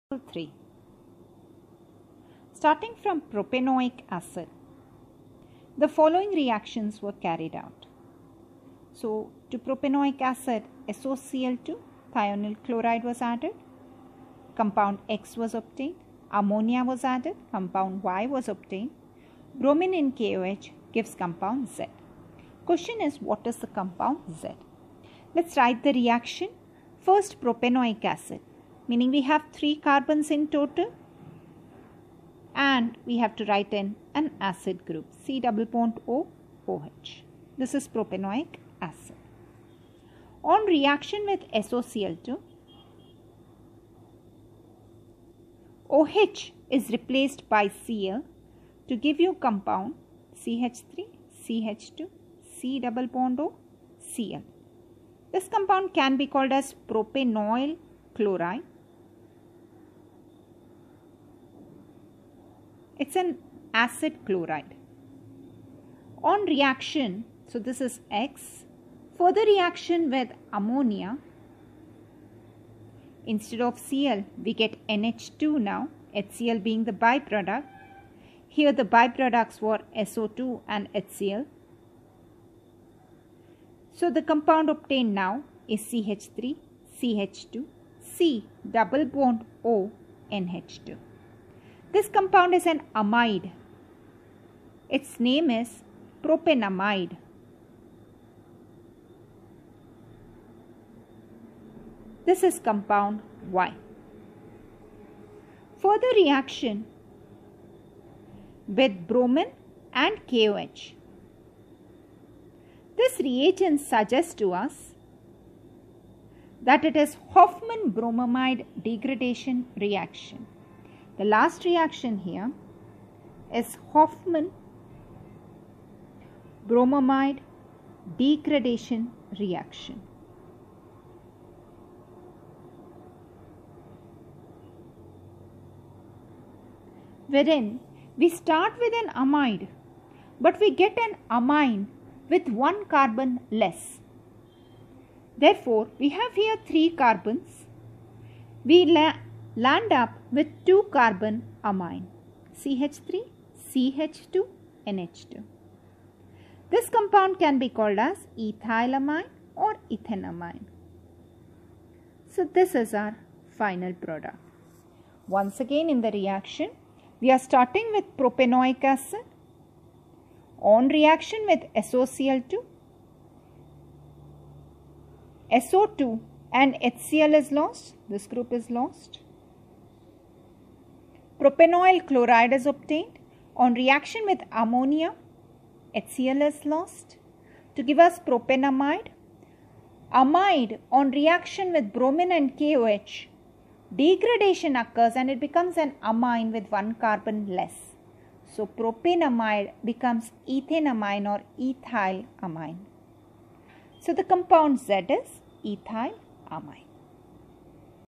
PU 2 | NEET S2 | Chemistry | Carboxylic Acids | Lecture 3 | Slide 43 | EX 3
Welcome to Concept Videos for NEET! This slide will explain Example 3 of Carboxylic Acids. We are sure you will have an effective learning experience.